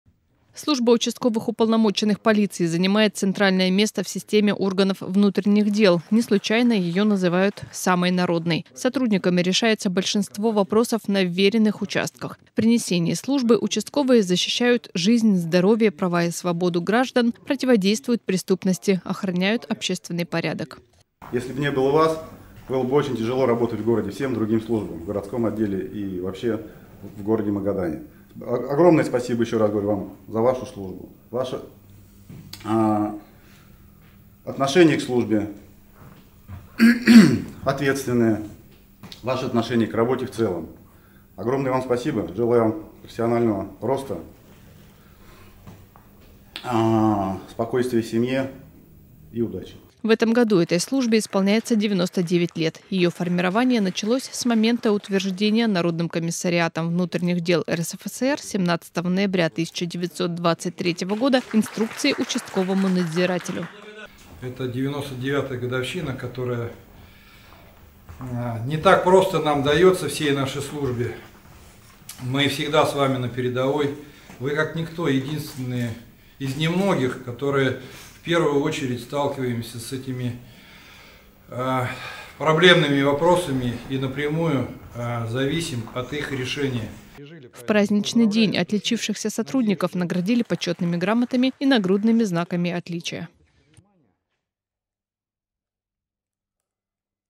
В Магадане отметили День участковых уполномоченных полиции
Торжественное мероприятие провели в горотделе